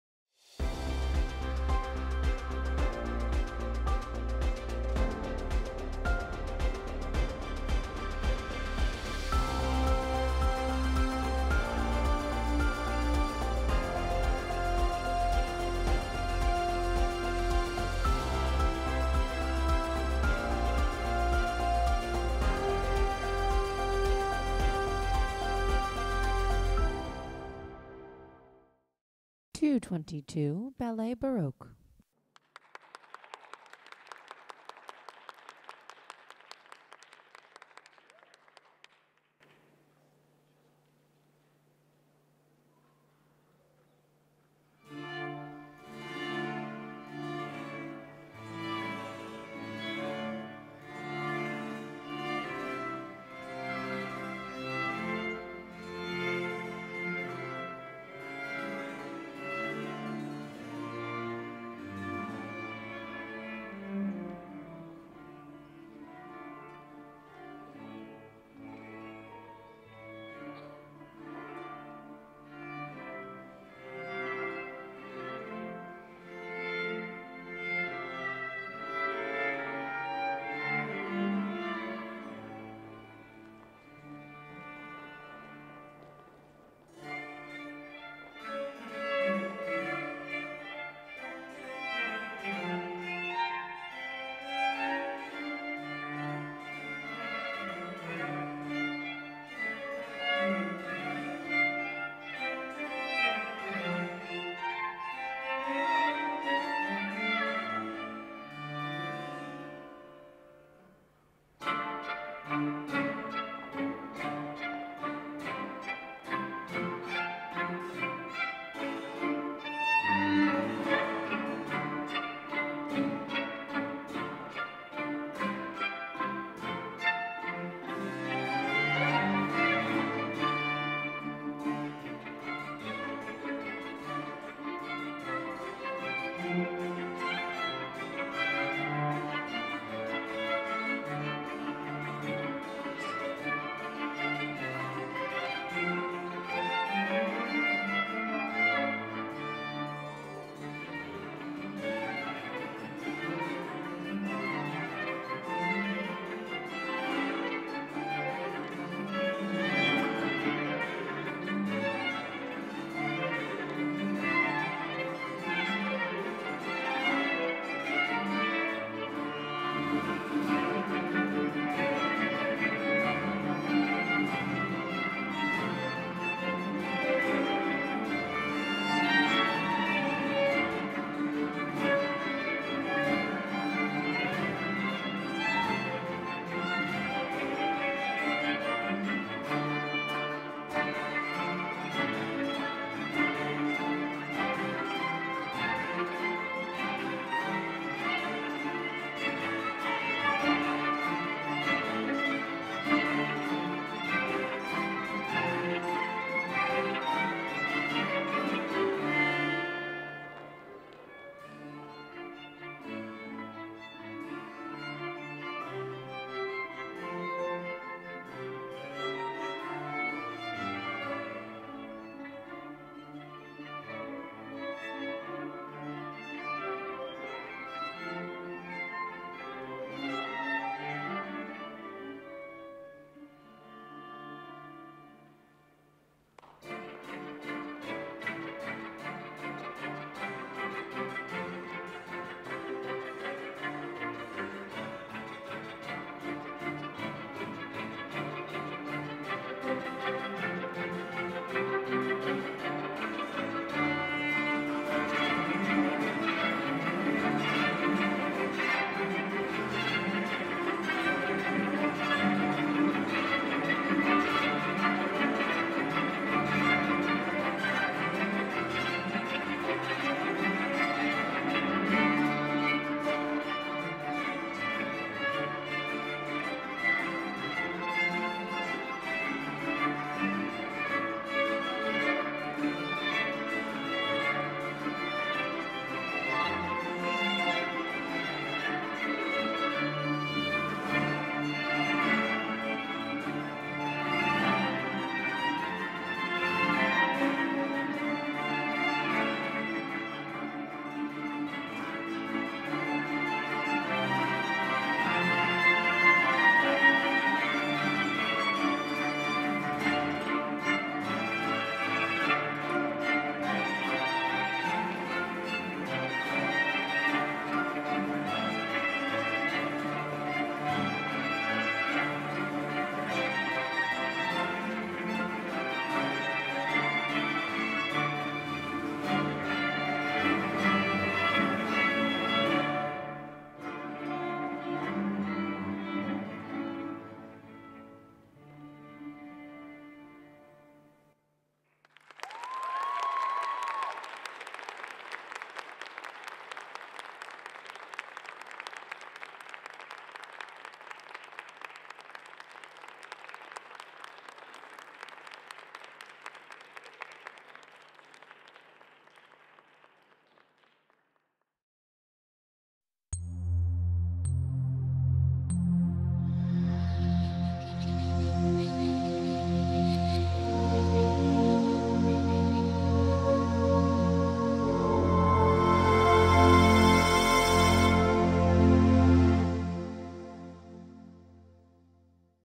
Caulfield School of Dance: Half Day Classical Ballet "Ballet Baroque"
Students of Caulfield School of Dance - of Port Moody, BC, Canada perform "Ballet Baroque" at the Youth America Grand Prix, Semi Finals, Las Vegas, Nevada. February 2018.
Choreography by Jocelyn Wozencroft.

Caulfield School of Dance, Port Moody, BC, Canada